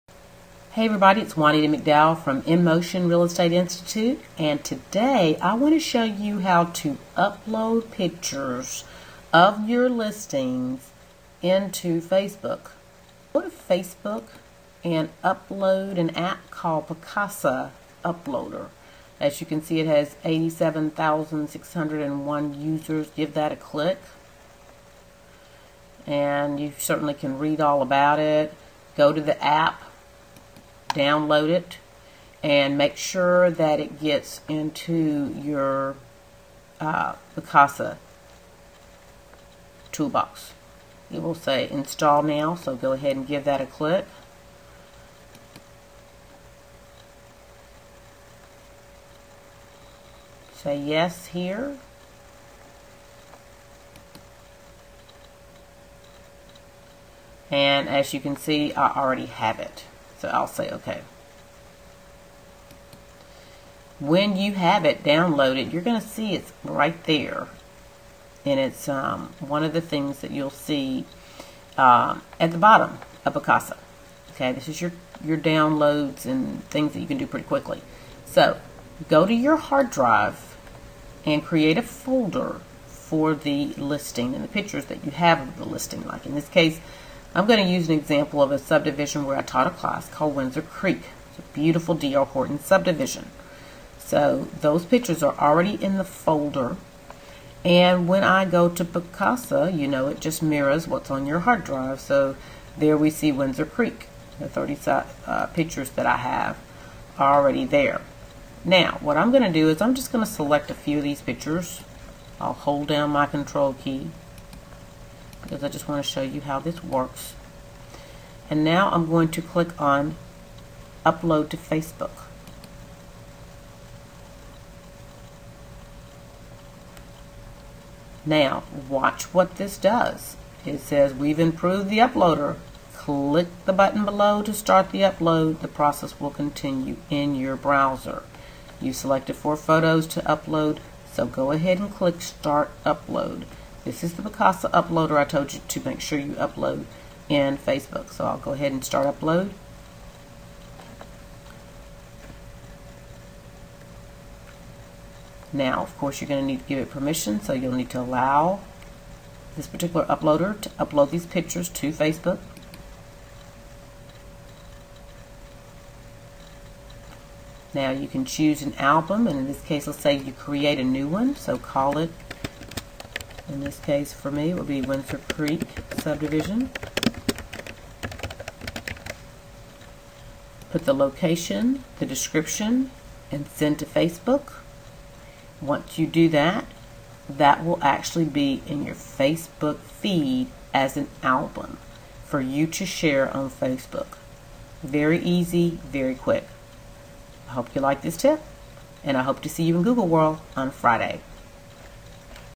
How to move photos from Picasa to Facebook
| Lead trainer Juanita McDowell answers a question from a student on how to take photos from your hard drive and quickly upload them to Facebook.